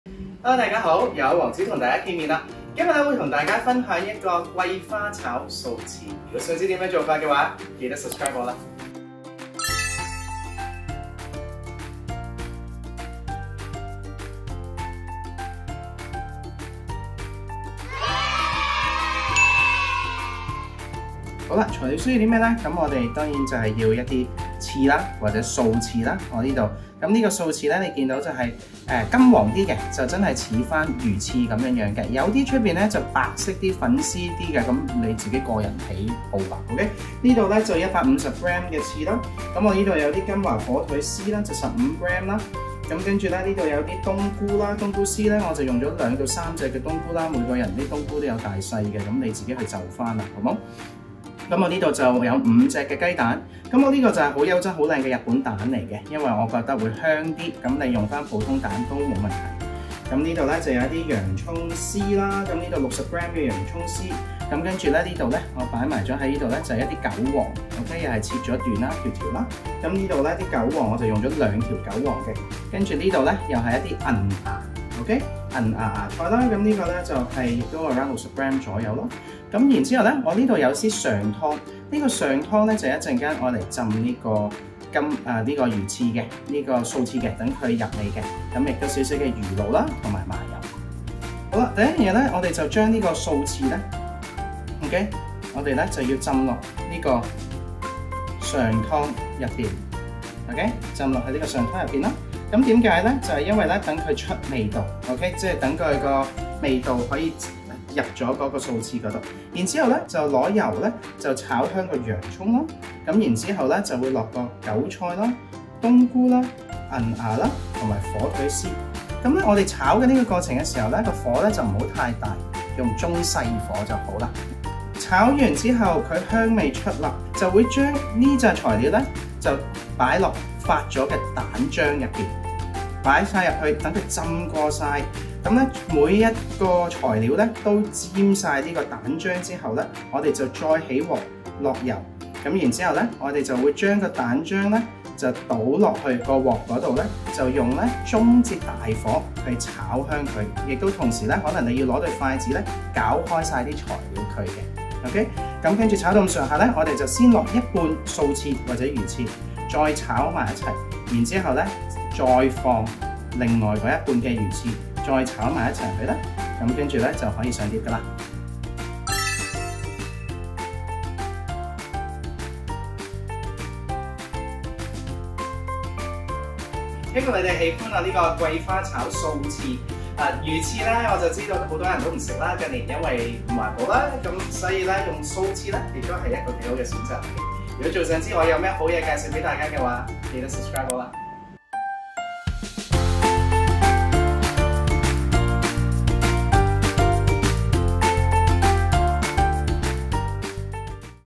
【王子煮場】桂花炒素翅 清爽營養又不失味道｜魚翅｜素食｜食譜
近日溫度不斷攀升，在家裏享用一道清爽又美味的桂花炒素翅再適合不過。這道菜以素翅為基底，綴以桂花的芬芳香氣，素翅則是用豆腐皮製成的素食替代品，質地鮮嫩有嚼勁。

即睇更多【生活Power Up】影片（包括烹飪、大灣區等），立即click入：